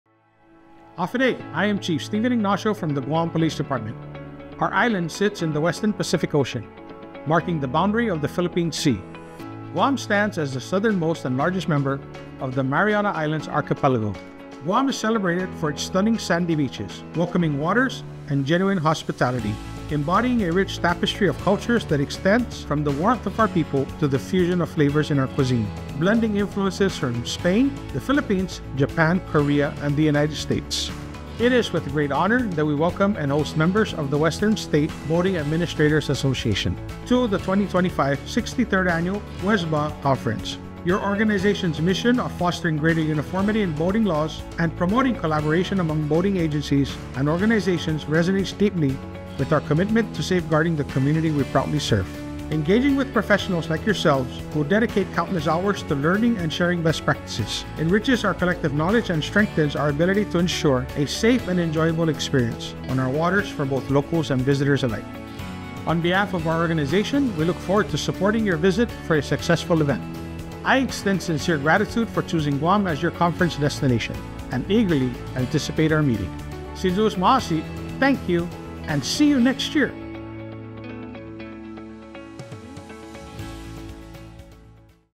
63rd Annual WSBAA Conference | Guam 2025
Voyage to the beautiful island of Guam for the 2025 63rd Western States Boating Administrators Association Annual Conference. Visit wsbaaguam.com for details.